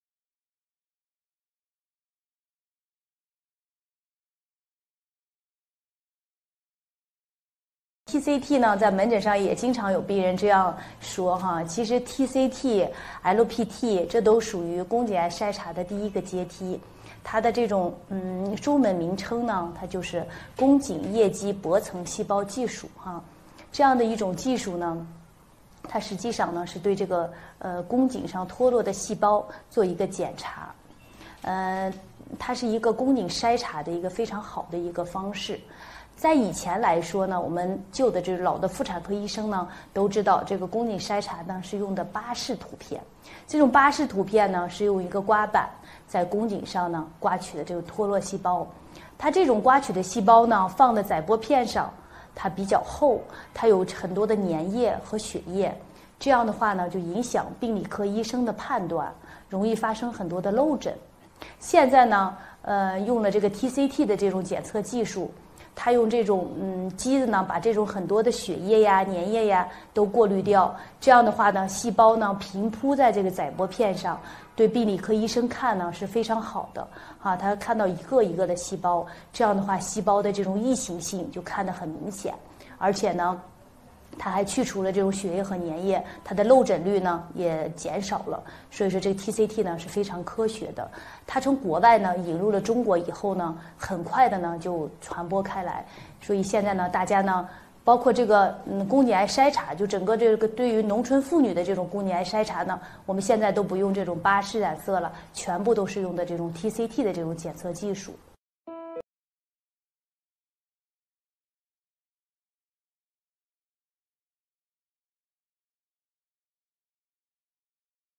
TCT是什么 赵卉 太原市妇幼保健院
TCT的中文名称是宫颈液基薄层细胞技术。这种技术实际上是对宫颈上脱落的细胞做一个检查，它是宫颈筛查非常好的一个方式。在以前的妇查科医生都知道宫颈筛查是用的巴氏涂片，这种巴氏涂片是一种刮板，在宫颈上刮落脱落细胞，把刮取得细胞放在载玻片上比较厚，它有很多的粘液和血液，会影响病理科医生的判断，容易发生漏诊。现在用的TCT这种检测技术，它是用机子把很多血液、粘液都过滤掉，这样细胞平铺在载玻片上，对病理科医生是非常好的，可以看到一个个的细胞，这样细胞的这种异型性就看的很明显，而且还祛除了这种血液和粘液，漏诊率也减少了。现在的宫颈癌筛查全部都是用的TCT检测技术。

What is TCT
The Chinese name of TCT is cervical fluid based thin layer cell technology. The technique is actually a test of cells that have fallen off the cervix, It's a great way to screen the cervix. In the past, gynecologists knew that cervical screening was a Pap smear, This pap smear is a kind of scraper, Scrape off the exfoliated cells on the cervix, The scraped cells are thicker on the slide, It has a lot of mucus and blood, It will affect the judgment of the pathologist, It is easy to miss diagnosis. Now TCT is the detection technology, It uses a machine to filter out a lot of blood and mucus, So the cells are laid flat on the slide, It's very good for pathologists, You can see cells by cells, In this way, the heteromorphism of cells is obvious, It also removes the blood and mucus, The rate of missed diagnosis also decreased. Now the cervical cancer screening is all using TCT detection technology.